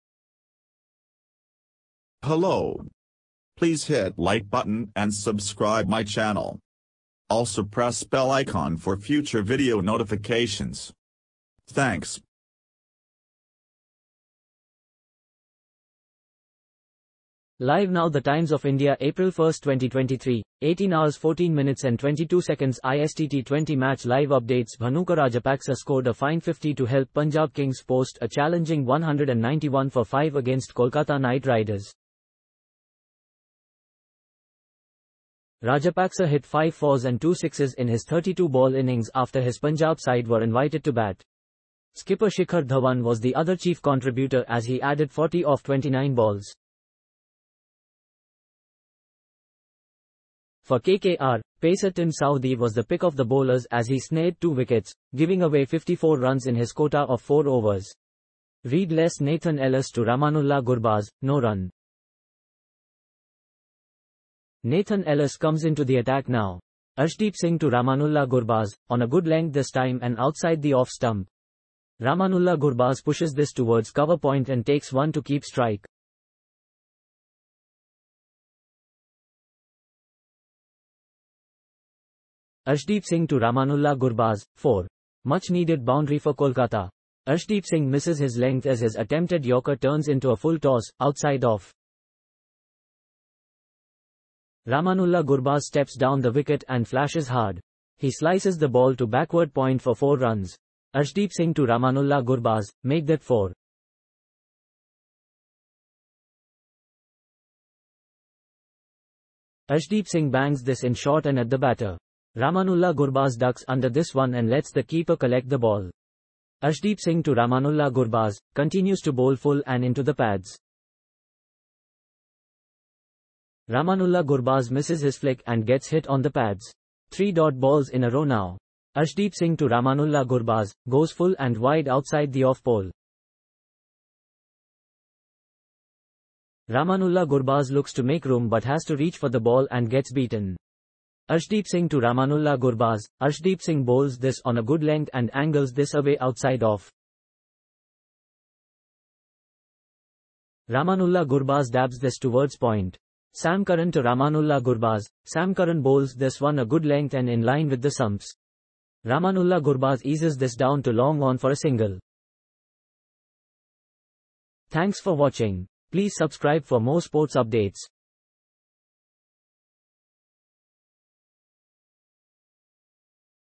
IPL Live Score: Punjab Kings vs Kolkata Knight Riders
LIVE NOW THE TIMES OF INDIA Apr 01, 2023, 18:14:22 IST T20 MATCH LIVE UPDATES Bhanuka Rajapaksa scored a fine fifty to help Punjab Kings post a challenging 191 for five against Kolkata Knight Riders. Rajapaksa hit five fours and two sixes in his 32-ball innings after his Punjab side were invited to bat. Skipper Shikhar Dhawan was the other chief contributor as he added 40 off 29 balls. For KKR, pacer Tim Southee was the pick of the bowlers as he snared two wickets, giving away 54 runs in his quota of four overs. Read Less Nathan Ellis to...